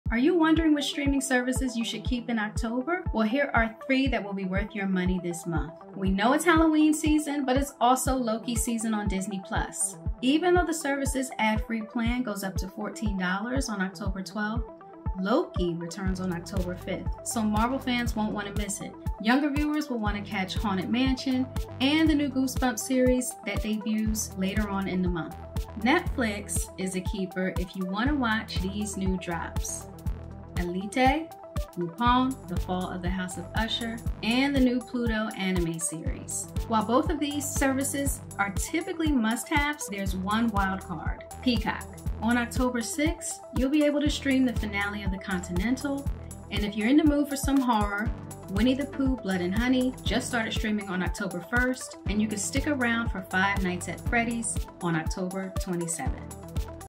Churn Those Streaming Services Like Butter
Rotating between streaming services can save you money. Here are out top 3 to keep for October.